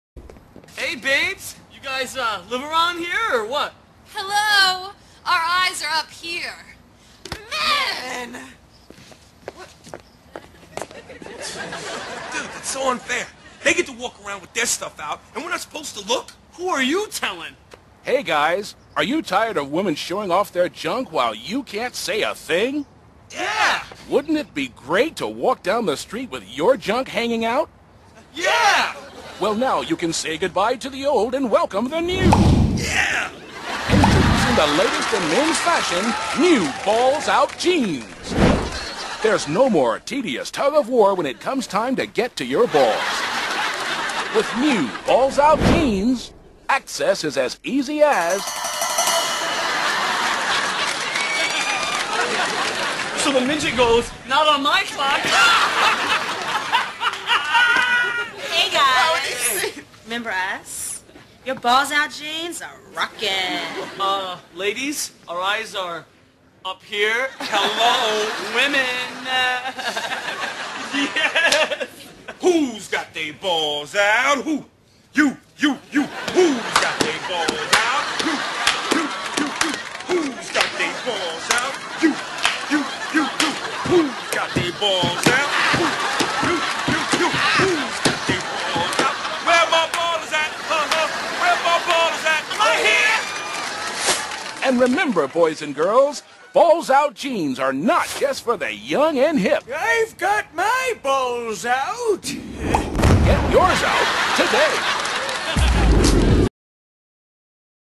new guy jeans (funny)
are you tired of women getting mad at you for staring at them. well heres your chance to get revenge!